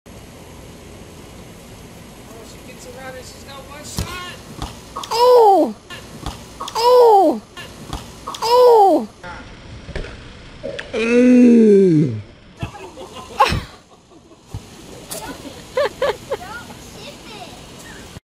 Save, with his face...
Keanu saves it with his face, kiyomi adds insult to injury.